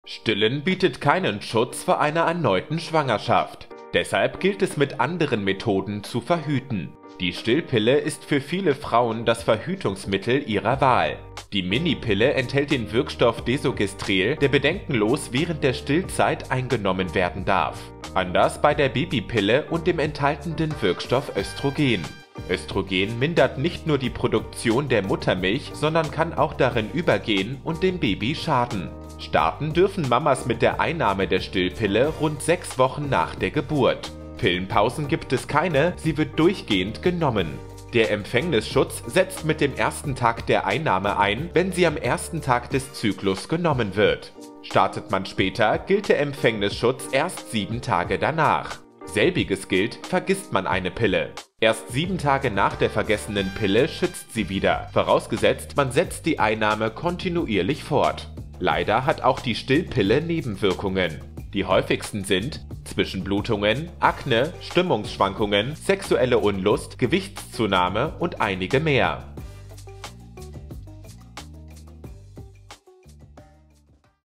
Stillpille: Was du zur Verhütung nach der Geburt wissen solltest
Die wenigsten Frauen denken kurz nach der Geburt über Sex nach. Doch irgendwann kehrt sie zurück, die Lust auf intime Zweisamkeit. Wer nicht direkt wieder schwanger werden will, sollte unbedingt verhüten, mit der Stillpille oder anderen Methoden. Wir klären auf, was ihr darüber wissen solltet.